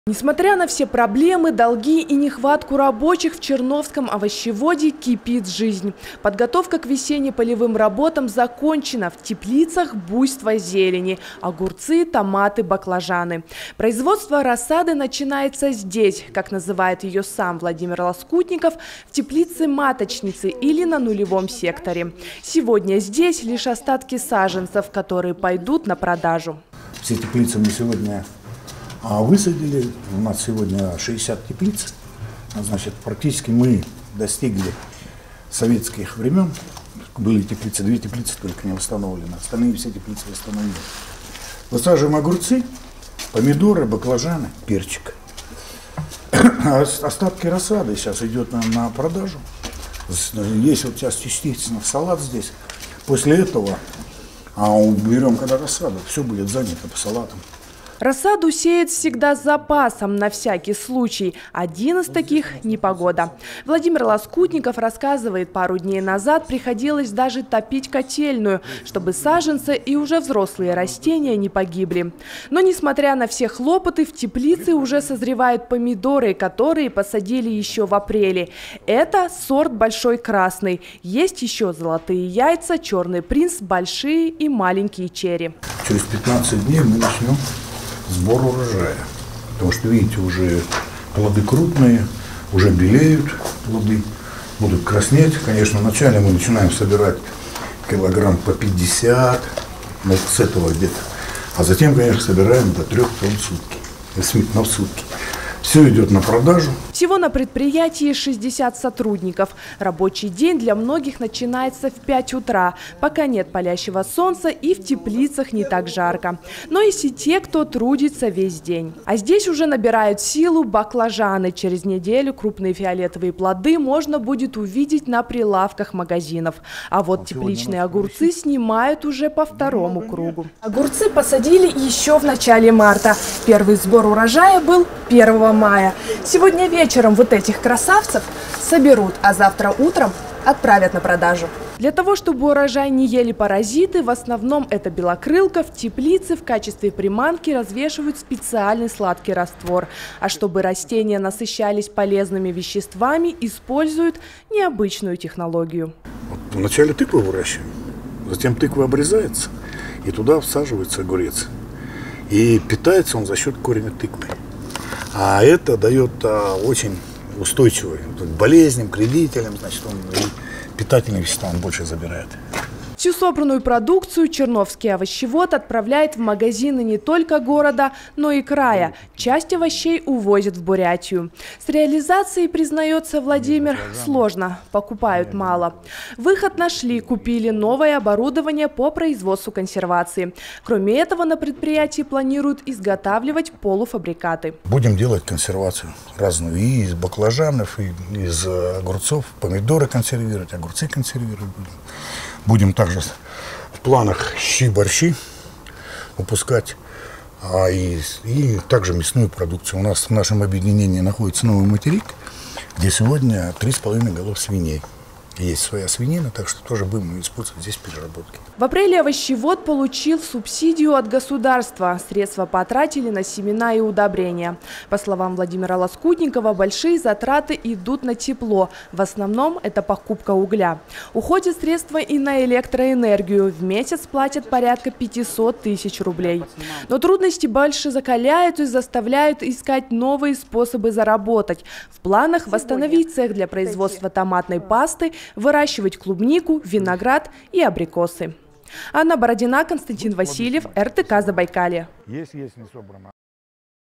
Буйство зелени
Угасающее несколько лет назад предприятие «Черновский овощевод» сегодня вновь расцветает. В теплицах буйство зелени – от огурцов до листьев салата, в цехах – новое оборудование, а в голове – большие планы. Как и чем сегодня живет предприятие, узнавали наши корреспонденты.